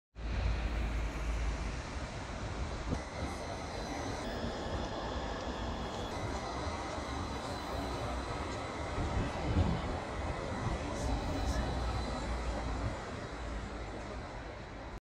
edinburgh trams
A tram in edinburgh going past at night time with none of the noise or ricketyness of a blackpool tram!